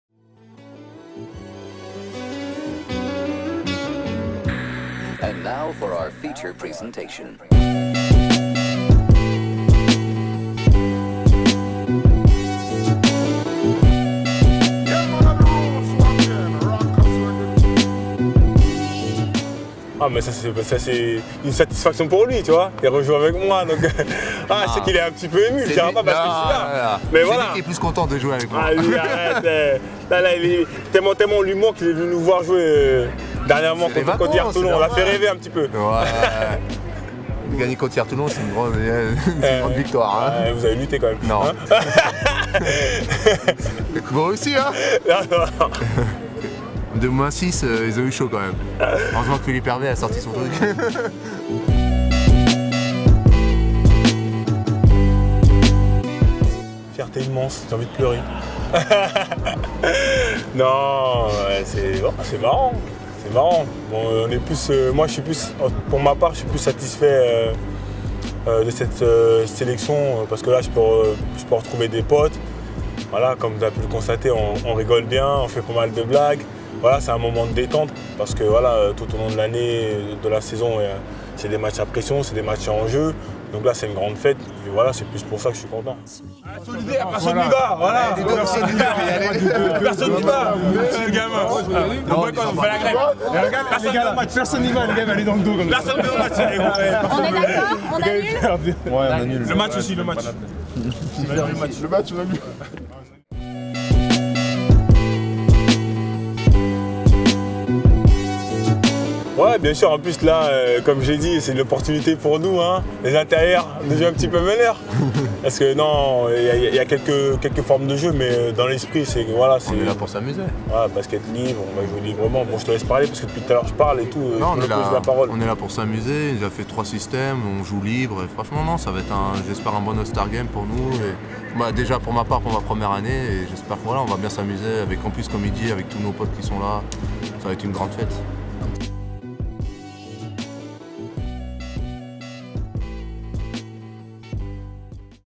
Adrien Moerman et Amara Sy se charrient au All Star Game
Entretien avec Amara Sy et Adrien Moerman, anciens coéquipiers à Orléans de nouveaux réunis lors du All Star Game 2011 à Bercy.